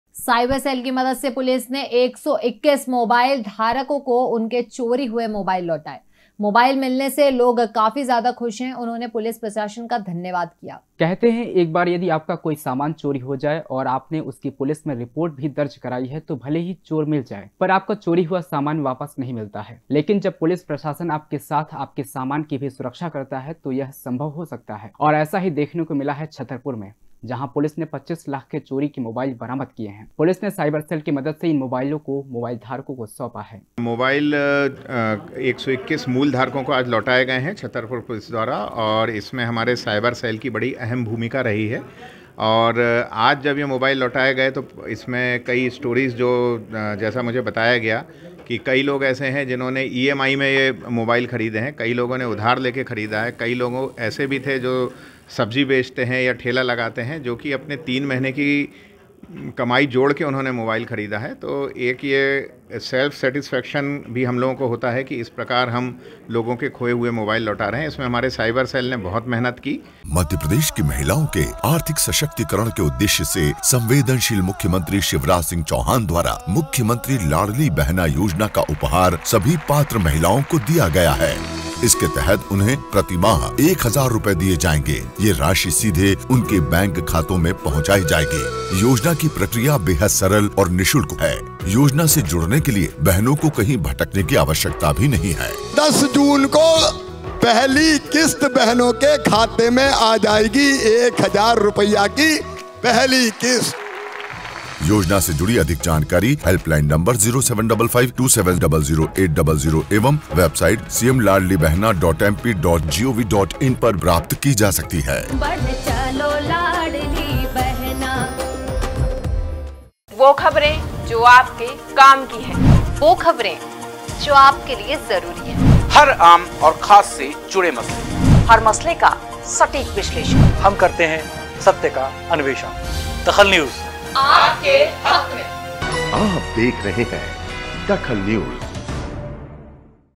DAKHAL NEWS पुलिस ने चोरी हुए पच्चीस लाख के मोबाईल जब्त किये
पुलिस ने चोरी हुए पच्चीस लाख के मोबाईल जब्त किये
पुलिस ने इन मोबाइलों को उनके मालिक को लौटाया